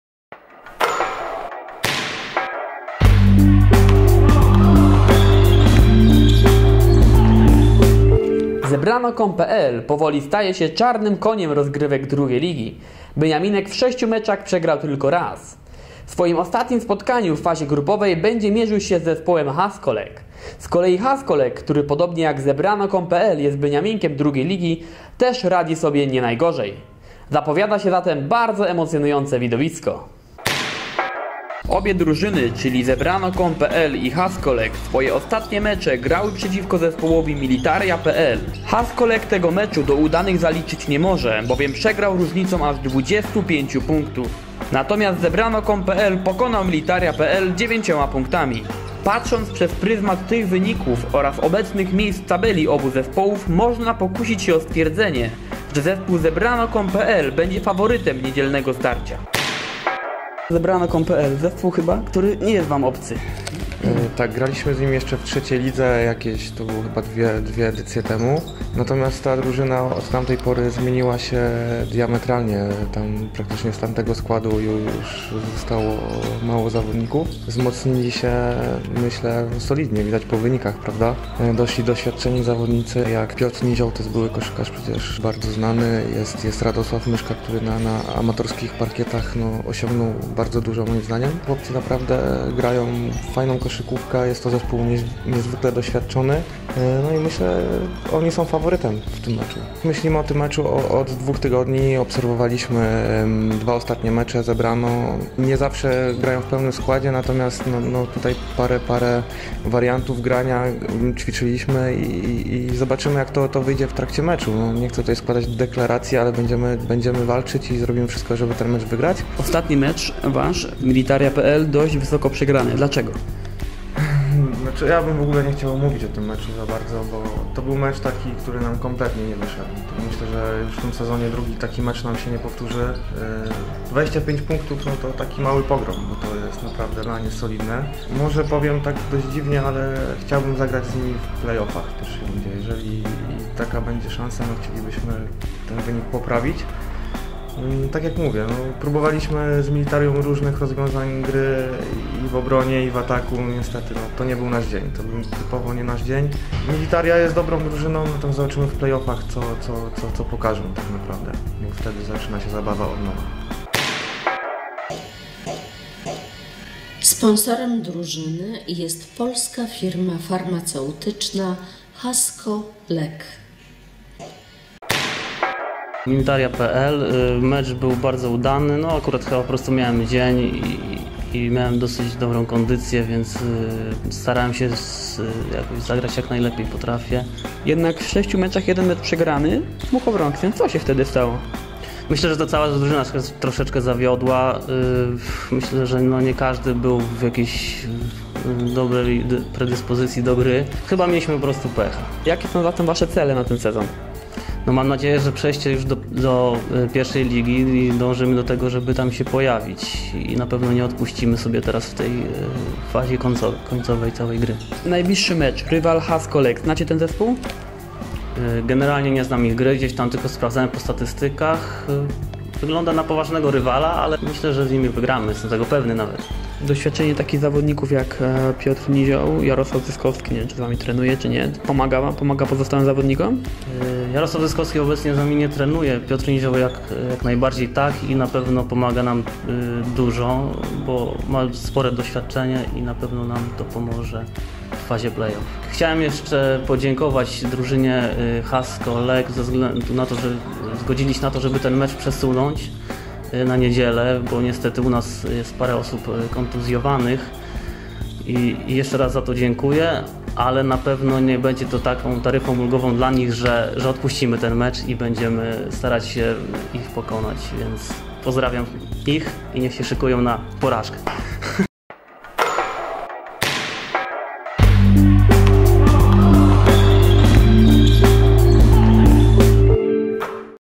Studio WRONBA TV, przed meczami 12. kolejki 33. sezonu
- Jestem przekonany, że wygramy z Hasco Lek! - Piotr Bieć (zebrano.com.pl, 2. liga). Studio WRONBA TV przed meczami 12. kolejki 33. sezonu.